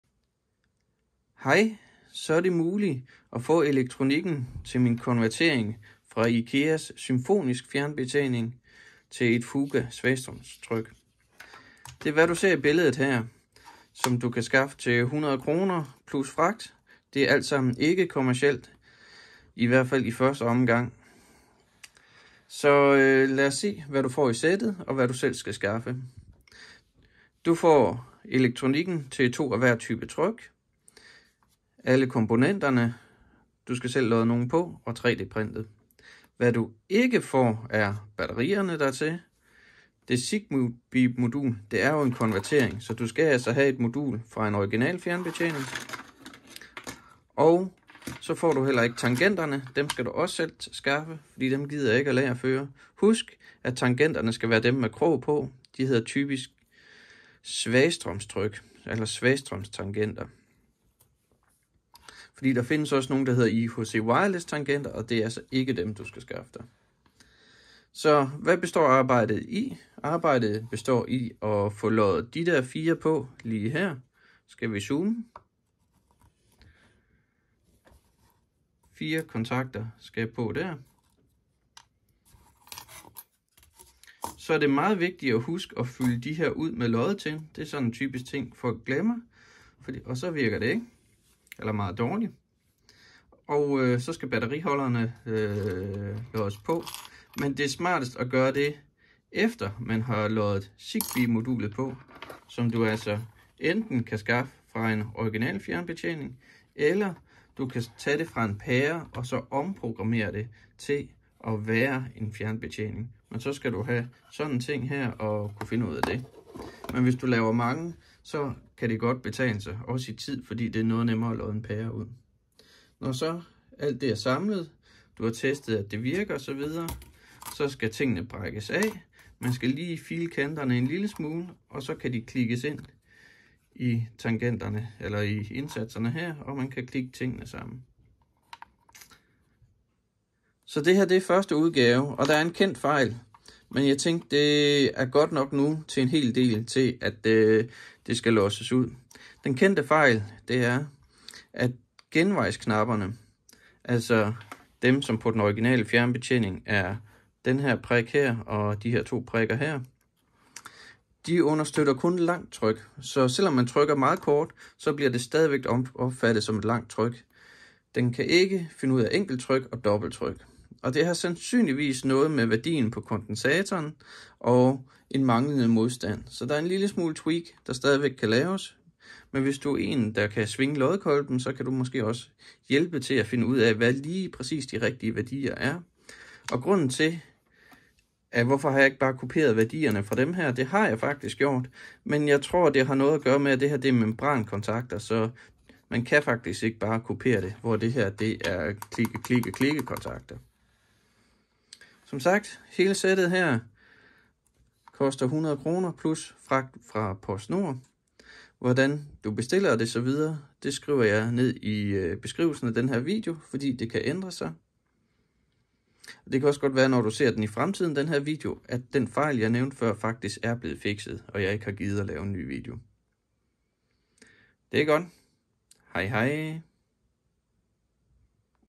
SYMFONISK til FUGA kit
Så det muligt at få gør-det-selv løsningen.

Priser, alt er til kostpris, ikke kommercielt:

    1 modul (1M), 20 DKK.
    1½ modul (1½M), 30 DKK.
    Fragt 50 DKK (kan afhentes i Aalborg).

    Hvor mange af 1M ønskes?
    Hvor mange af 1½M ønskes?
    Postadresse?
    Telenummer til Portnord og MobilePay?
    Email til Portnord?

Det vil være praktisk at bestille 2 x 1M og 2 x 1½M, da det er en hel plade. Altså samlet 100 DKK per plade.

Bemærk at denne første revision har en fejl, så kun langt tryk er tilgængeligt på de 2 genvejs-knapper. Det vil sige man ikke kan bruge genvejs-knapperne med DERIGERA hub, og kun langt tryk med IKEA gateway.

Kittet inkludere:

    Plade med 2 x 1M og 2 x 1½M version (hvis du ønske begge typer kontakter).
    Alle elektronik-komponenter, undtagen ZigBee modul.
    3D Printede indsatser.

Hvad du skal skal skaffe:

    ZigBee modul fra IKEA SYMFONISK fjernbetjening .
    Batterier.
    Svagstrømstangenter og midterbjælke.

Mere præcist så er det:

    4 x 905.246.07 eller MGM210L022JNF2 (ikke MGM210LA22JNF2).
    6 x CR2032.
    10 x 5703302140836 (ikke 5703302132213).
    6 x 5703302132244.

Hvad skal du udføre:

    Pålod 4 x skydeknapper.
    Klargør 4 x ZigBee modul.
    Pålod 4 x ZigBee modul.
    Fyld tin i 6 x batteri-ø.
    Pålod 6 x batteriholder.
    Programmer ZigBee modul, hvis nødvendigt.
    Test kontakten.
    Knæk de enkle kontakter fri.
    Fil knæk-punktet glat.
    Klink delene sammen.